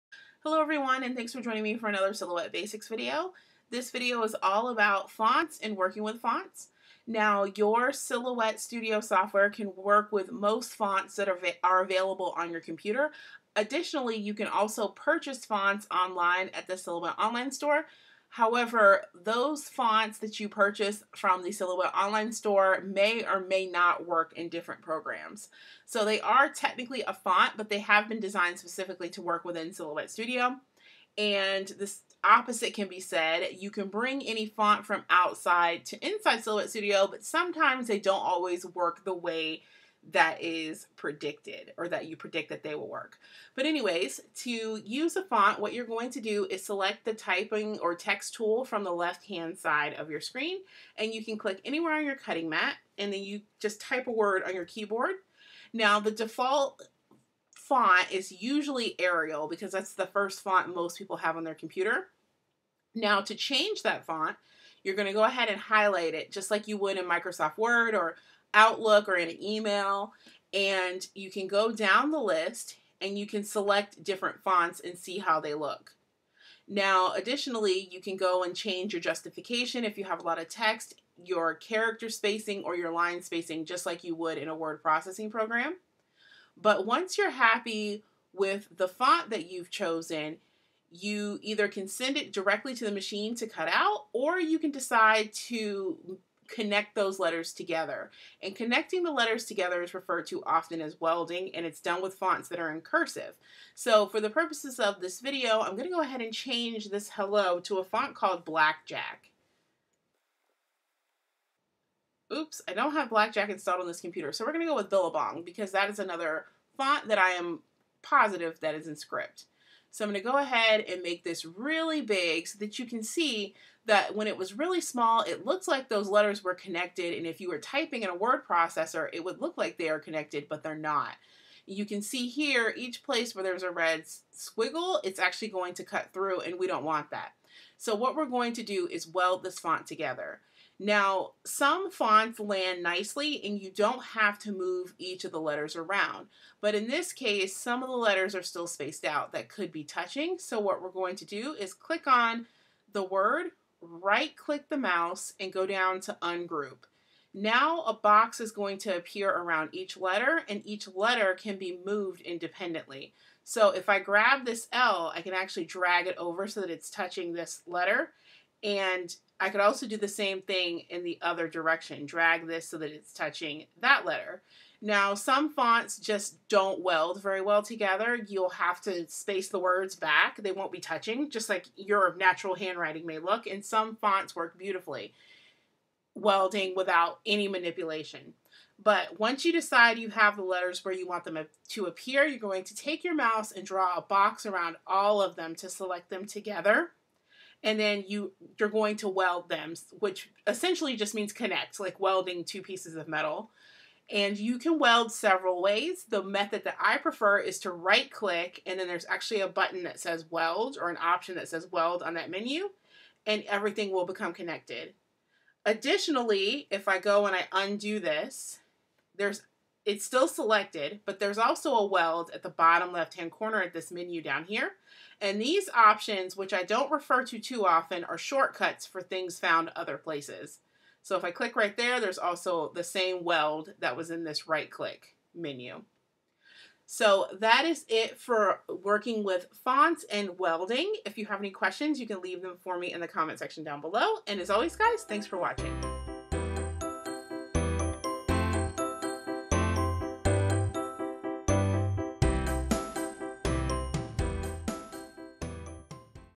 Using and Welding Fonts | Silhouette Basics
Welcome to Silhouette Basics!  Quick videos for the new Silhouette owner. This video shows the process of using fonts, changing font styles and welding!